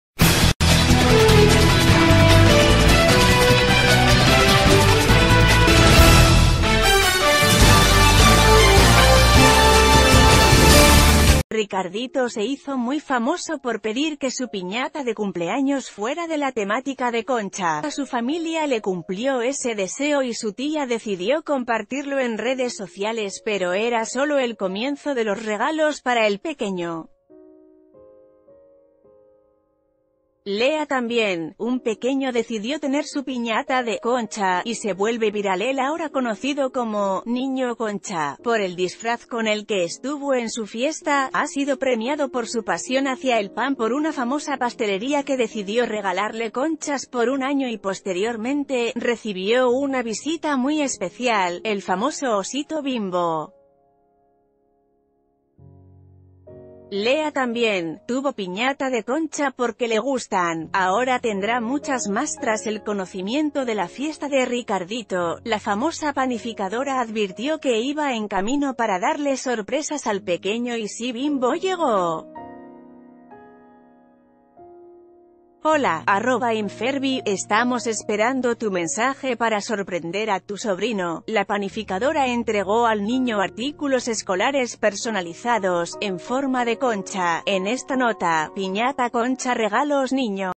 "Niño concha" recibe una visita muy especial y más regalos
Ricardito se hizo muy famoso por pedir que su piñata de cumpleaños fuera de la temática de concha, su familia le cumplió ese deseo y su tía decidió compartirlo en redes sociales, pero era solo el comienzo de los regalos para el pequeño. Lea también: Un pequeño decidió tener su piñata de “concha” y s...